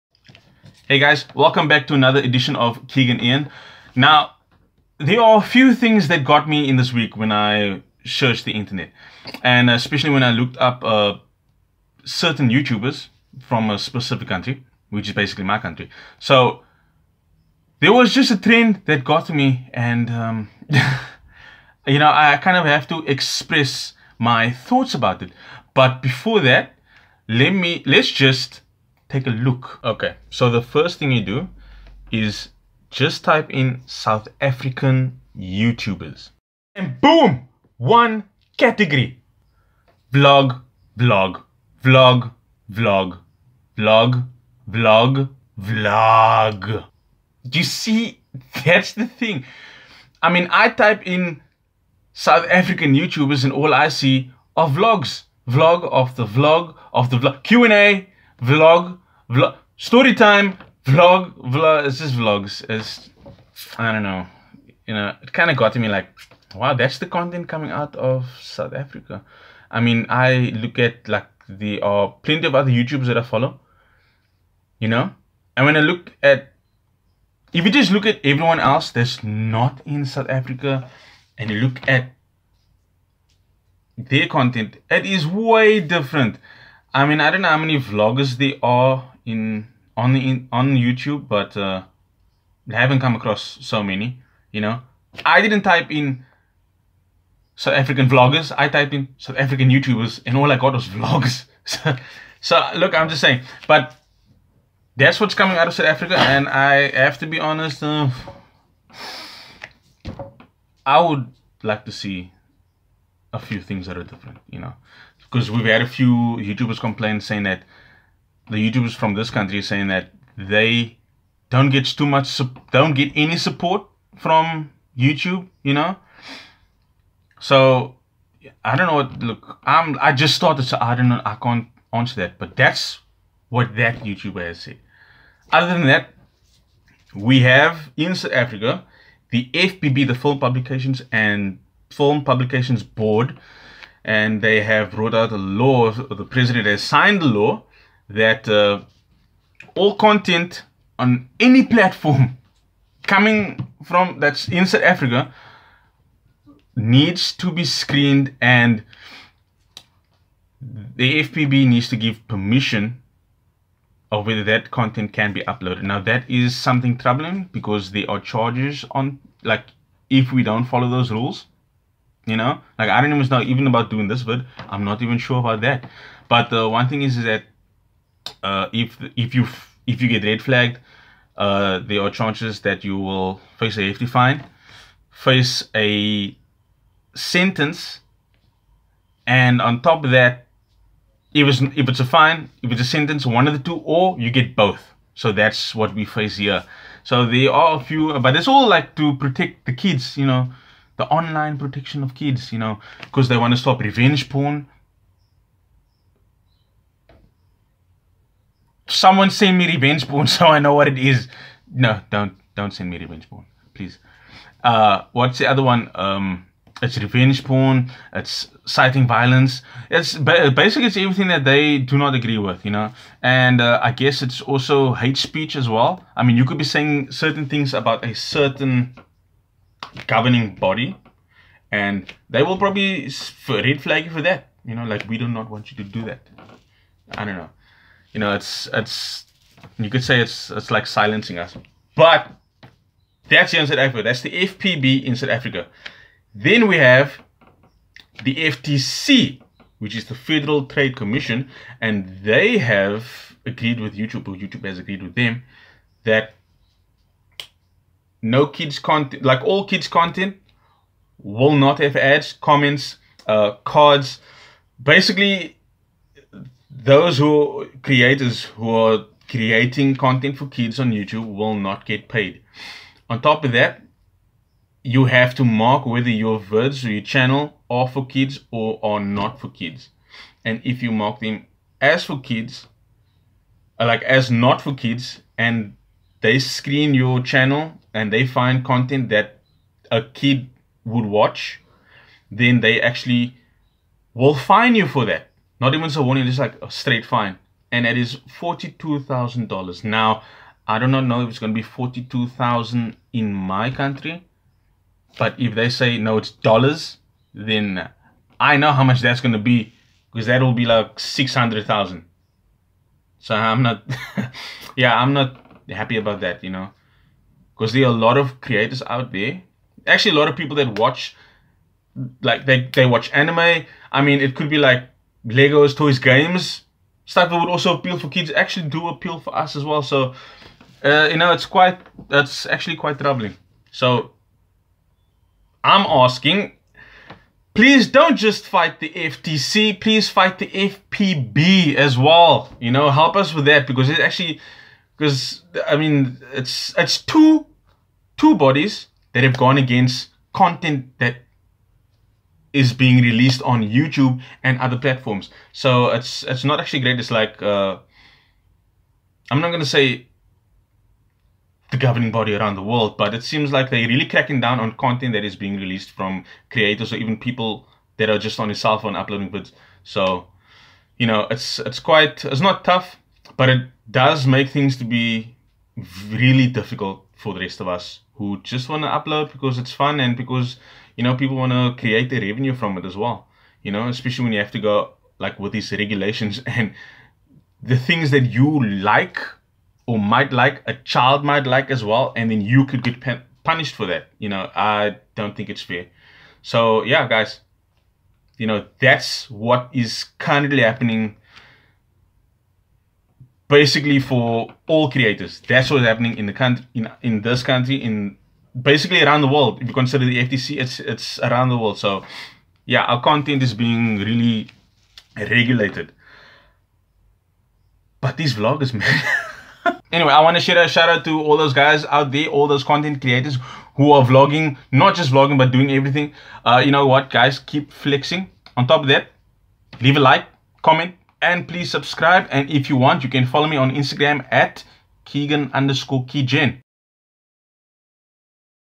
We need HELP!! FPB, FTC & VLOGS
While searching for content creators from my nation I couldn't help but notice a TREND in content being uploaded from them, but there are worse things than vloggers apparently.  Commentary | Reactions | South African